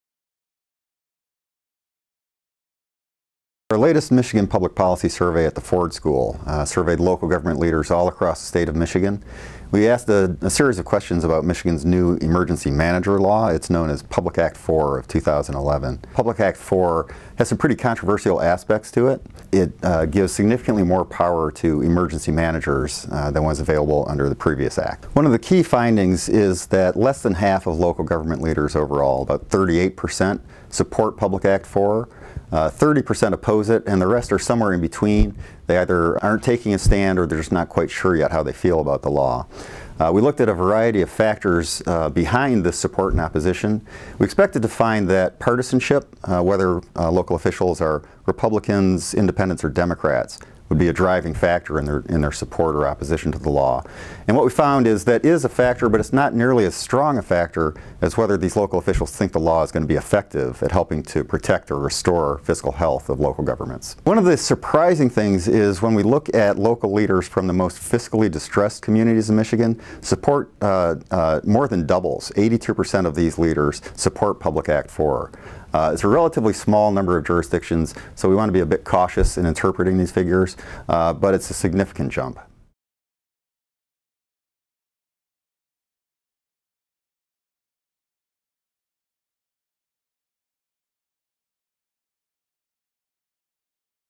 Thomas Ivacko: Local leaders' opinions split over state's emergency manager law
Thomas Ivacko is the administrator and program manager of the Ford School's Center for Local, State and Urban Policy. He oversees the Michigan Public Policy Survey program.